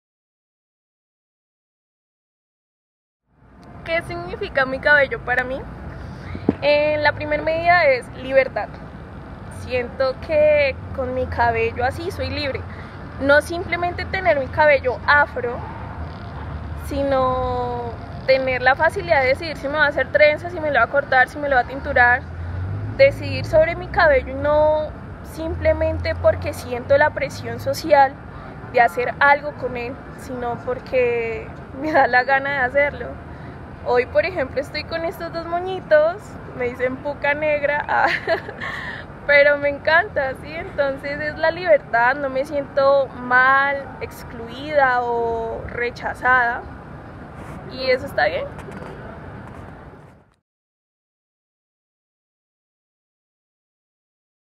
EPT: Karen Ortiz Que Significa tu Cabello Para Ti
Karen cuenta a la comunidad EPT, que significa su cabello actualmente para ella. Si quieres conocer una anécdota de pelos de Karen sigue este enlace